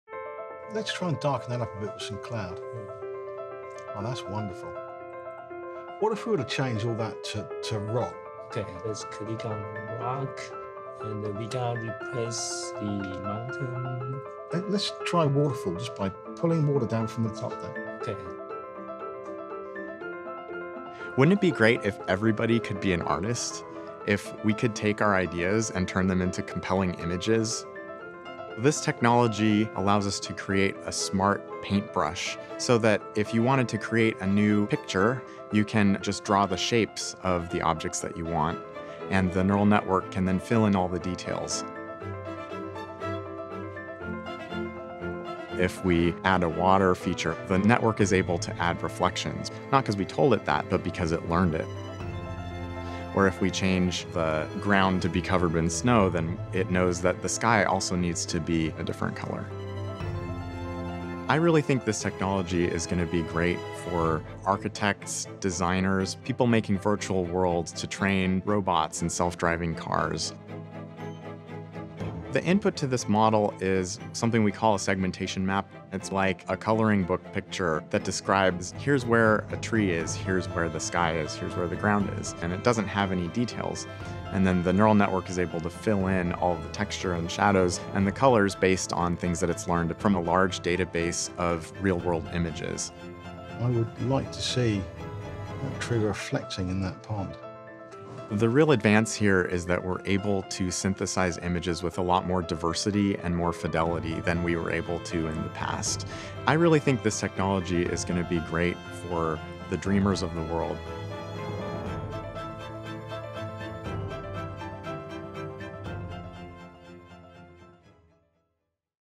GauGAN: Changing Sketches into Photorealistic Masterpieces
A deep learning model developed by NVIDIA Research turns rough doodles into highly realistic scenes using generative adversarial networks (GANs). Dubbed GauGAN, the tool is like a smart paintbrush, converting segmentation maps into lifelike images.